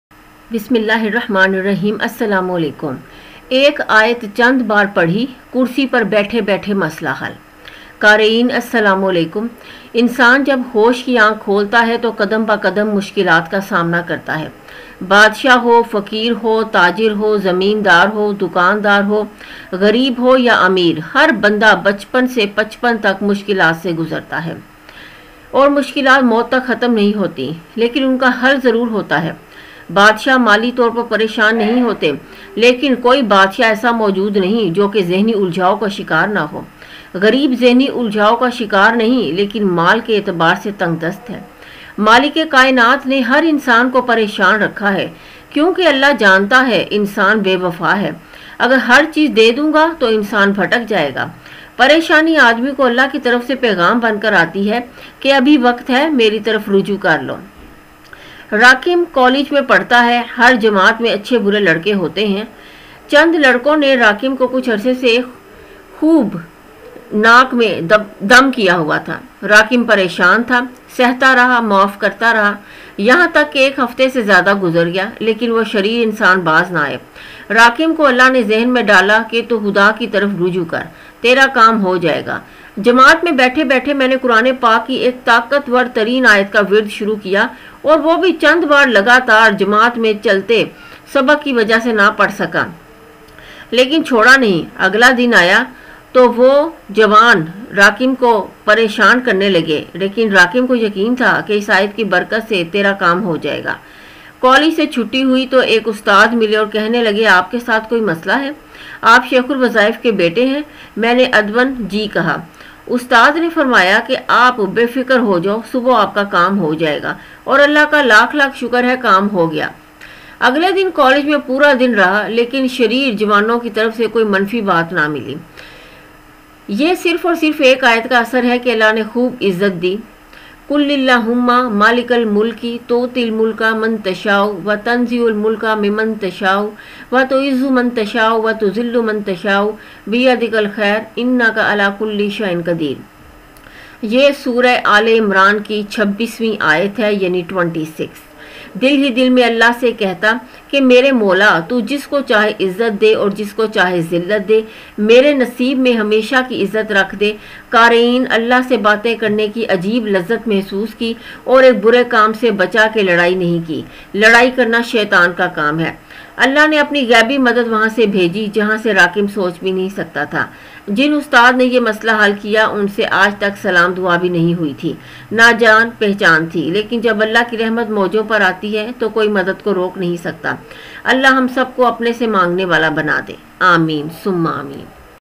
Amal to Solve Problem Easily
In this video ayat 26 of Surah Aleimran is explained to solve problems easily.